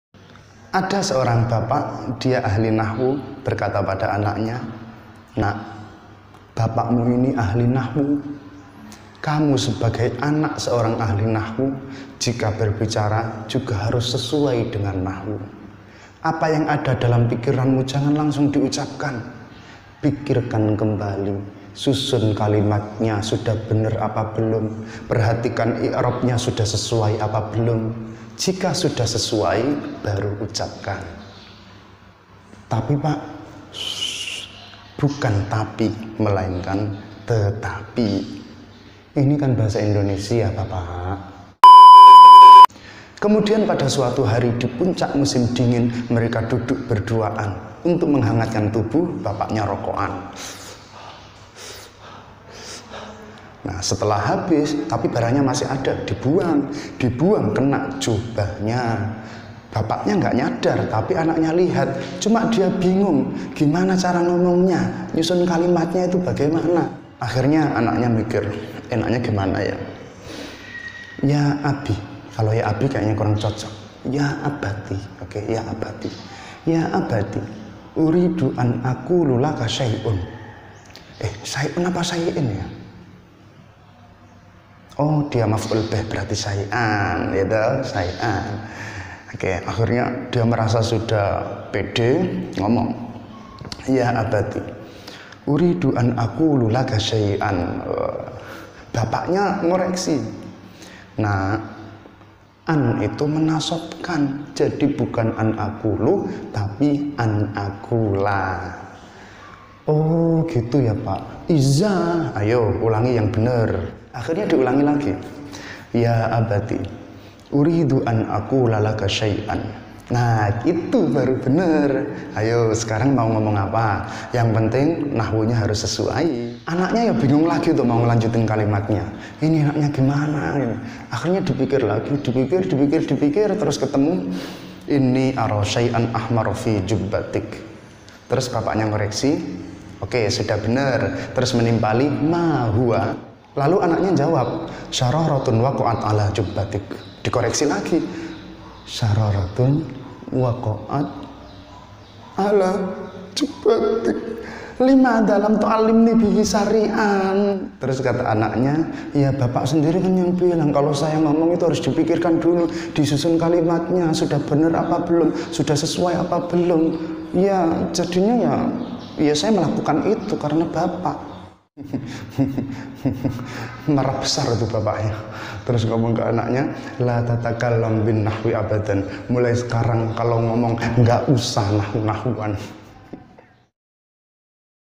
Jangan Berbicara Pakai Nahwu - Humor Lucu Ulama Nahwu
Di beberapa pesantren, ada yang mengharuskan para santri untuk menggunakan bahasa Arab dalam percakapan sehari-hari. Bahkan, diberlakukan hukuman bagi yang tidak memakai bahasa Arab.

Namun, hukuman itu hanya berlaku jika si santri tidak menggunakan bahasa Arab. Tidak peduli bahasa Arabnya benar atau tidak. Sesuai nahwu atau tidak.

Mungkin, ini karena pesan seorang ahli nahwu kepada anaknya, untuk tidak menggunakan nahwu saat berbicara.